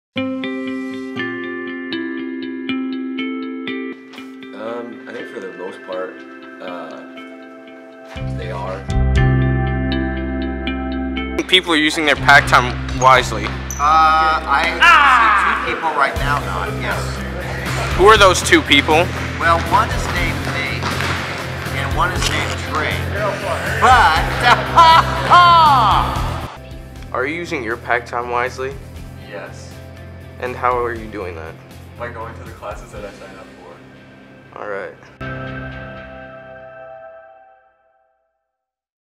Pack promo by Trey Andrews
Encouraging students how to us PACK time the right way.
BHHS iVideo class. Trey Andrews